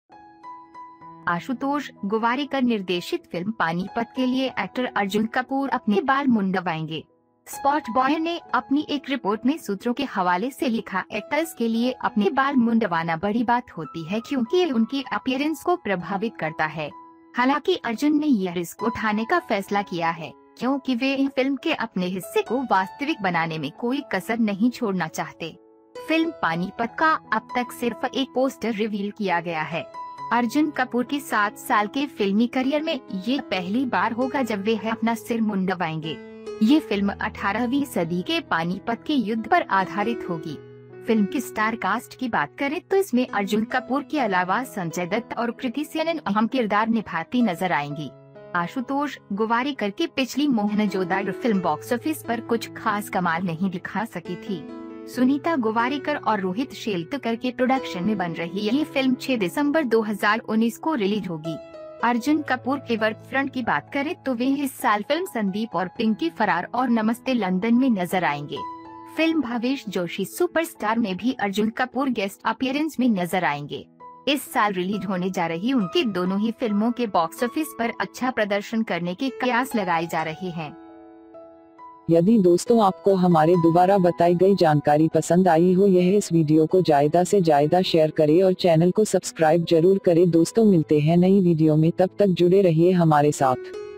7 साल के फिल्मी करियर में अर्जुन कपूर पहली बार करेंगे ये काम जानकर चौक जायेगे आप
7 साल के फिल्मी करियर में अर्जुन कपूर पहली बार करेंगे ये काम जानकर चौक जायेगे आप ...

आशुतोष गोवारिकर निर्देशित फिल्म पानीपत के लिए एक्टर अर्जुन कपूर अपने बाल मुंडवाएंगे. स्पॉटबॉय ने अपनी एक रिपोर्ट में सूत्रों के हवाले से लिखा- एक्टर्स के लिए अपने बाल मुंडवाना बड़ी बात होती है क्योंकि यह उनकी अपीयरेंस को प्रभावित करता है. हालांकि अर्जुन ने यह रिस्क उठाने का फैसला किया है क्योंकि वह फिल्म के अपने हिस्से को वास्तविक बनाने में कोई कसर नहीं छोड़ना चाहते. फिल्म पानीपत का अब तक सिर्फ एक पोस्टर रिवील किया गया है. अर्जुन कपूर के 7 साल के फिल्मी करियर में यह पहली बार होगा जब वह अपना सिर मुंडवाएंगे. यह फिल्म 18वीं सदी के पानीपत के युद्ध पर आधारित होगी.
फिल्म की स्टार कास्ट की बात करें तो इसमें अर्जुन कपूर के अलावा संजय दत्त और कृति सेनन अहम किरदार निभाती नजर आएंगी. आशुतोष गोवारिकर की पिछली मोहनजोदाड़ो फिल्म बॉक्स ऑफिस पर कुछ खास कमाल नहीं दिखा सकी थी. सुनीता गोवारिकर और रोहित शेल्तकर के प्रोडक्शन में बन रही यह फिल्म 6 दिसंबर 2019 को रिलीज होगी.अर्जुन कपूर के वर्क फ्रंट की बात करें तो वह इस साल फिल्म संदीप और पिंकी फरार और नमस्ते लंदन में नजर आएंगे. फिल्म भावेश जोशी सुपरस्टार में भी अर्जुन कपूर गेस्ट अपीयरेंस में नजर आएंगे. इस साल रिलीज होने जा रही उनकी दोनों ही फिल्मों के बॉक्स ऑफिस पर अच्छा प्रदर्शन करने के कयास लगाए जा रहे हैं.